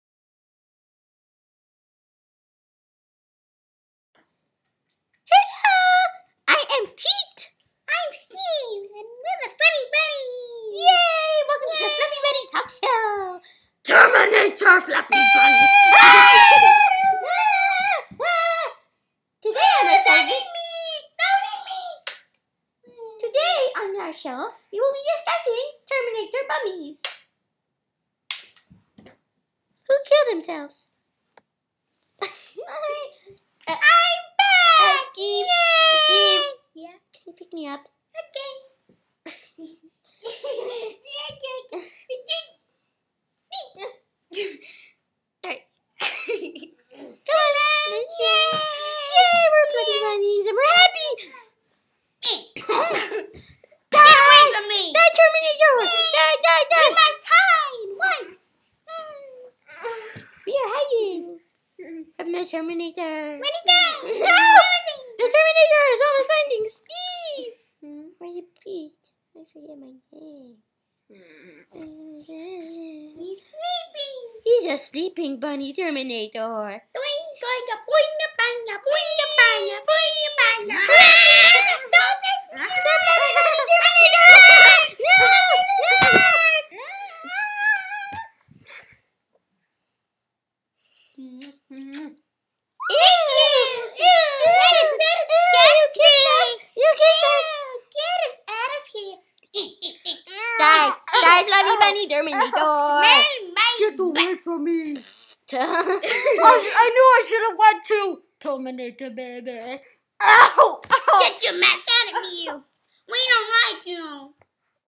Fluffy Bunny Terminator Attacks! AHH!
Will the Bunnies prevail? Or is this the beginning of the end for Steve and Pete?